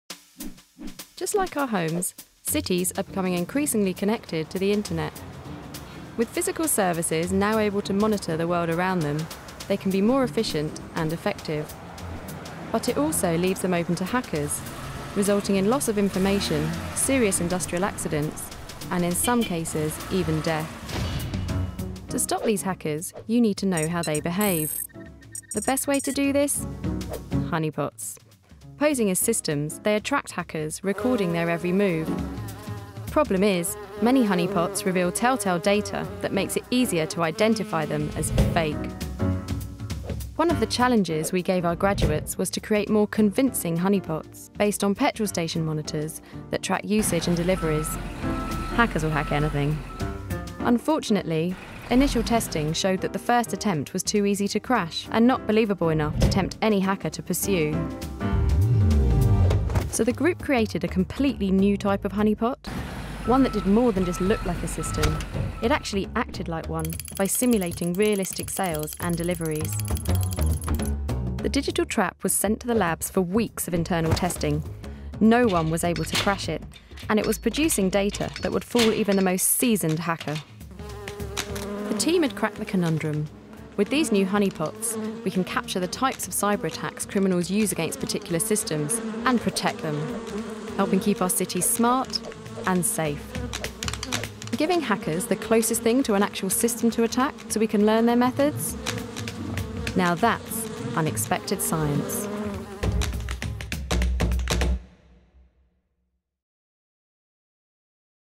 Unexpected Science | Creating honeypots to fight cyber attacks
Dstl graduates have created highly realistic fake online systems called Honeypots to tempt hackers and discover their methods to help fight future cyber-attacks.